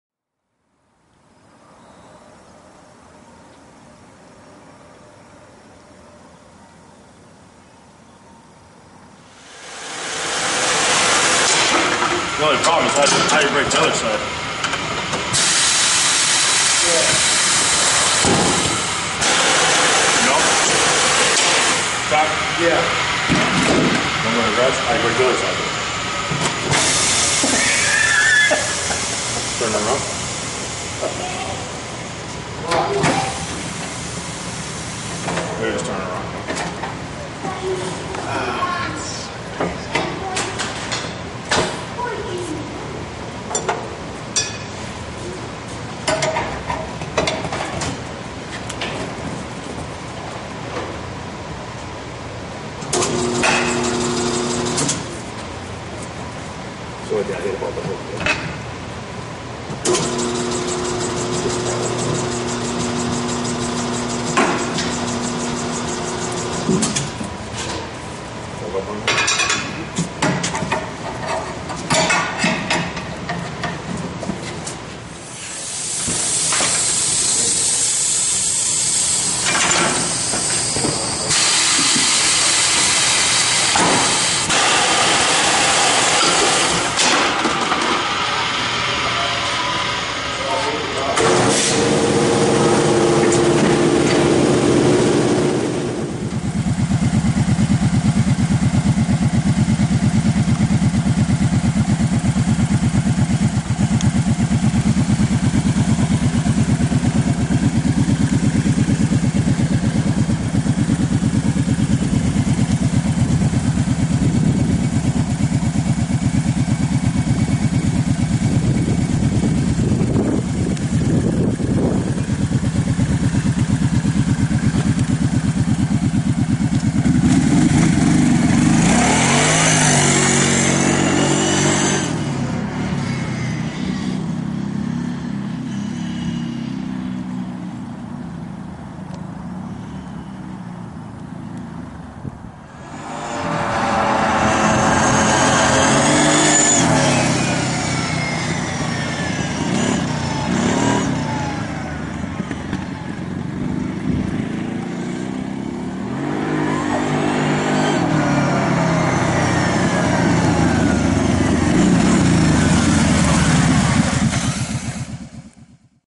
New ITP Rims-Kawasaki 650 prairie
Kawasaki 650 prairie 4X4 V-TWIN W/ ITP Rims and ITP Terracross tires